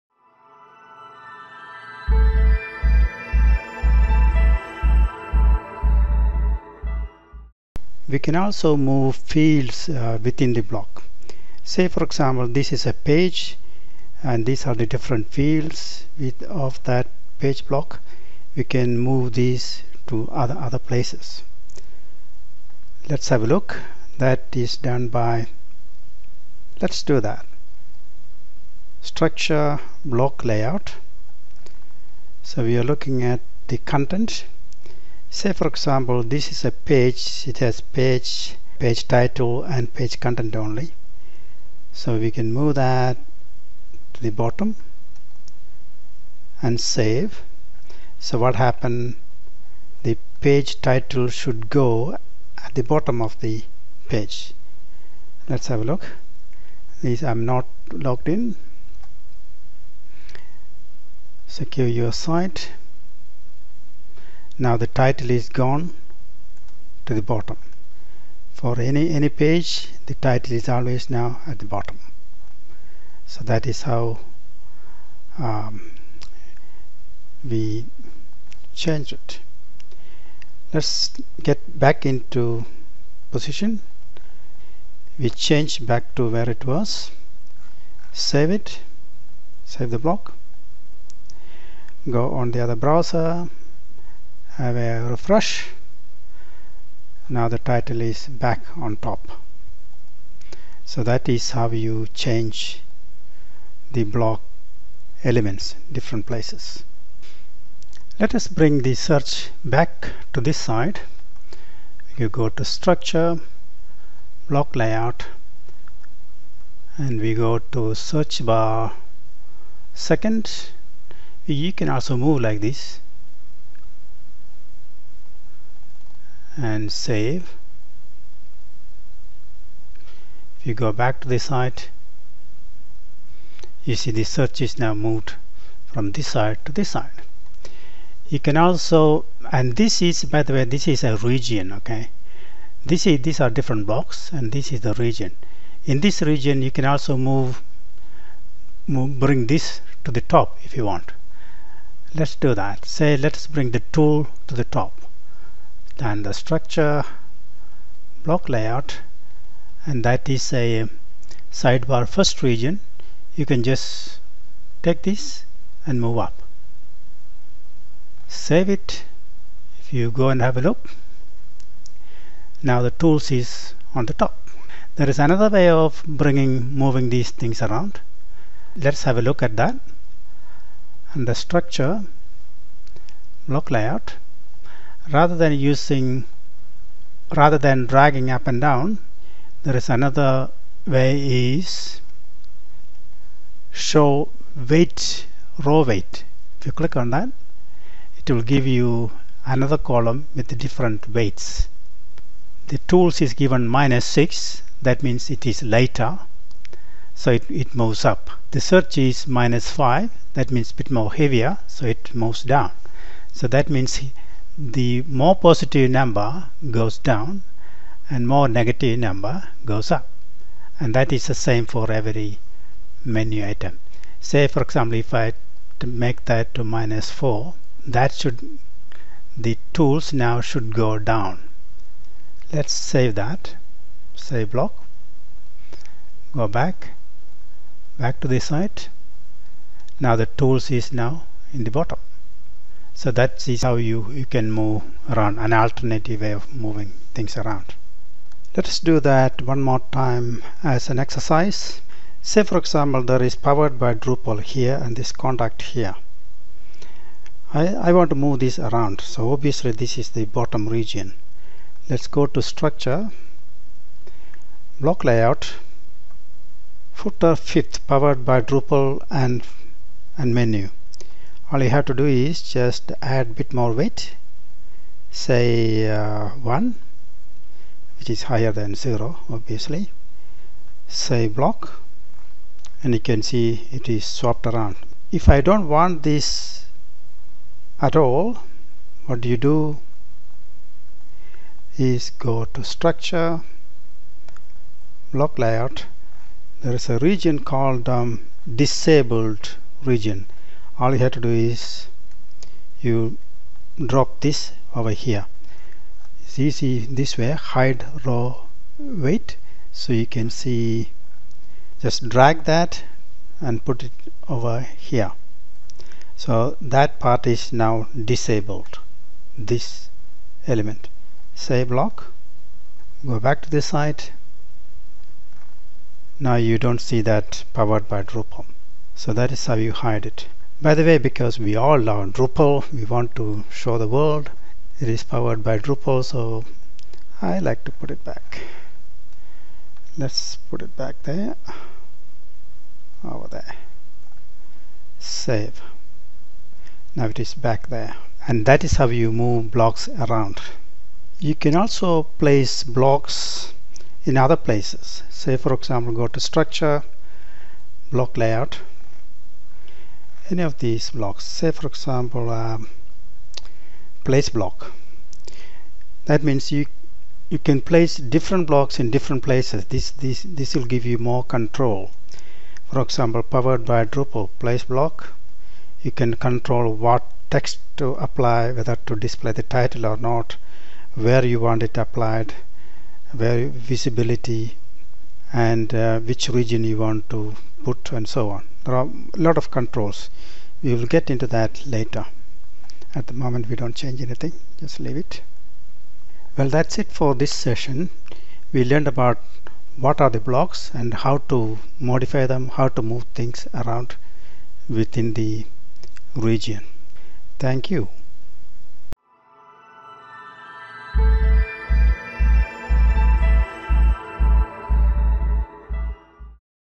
20 - Moving Elements Within Blocks
Drupal 8 Essential Training is for anyone who wants to use Drupal CMS in their organisation. These lessons are part of a beginner series, with over 60 lessons.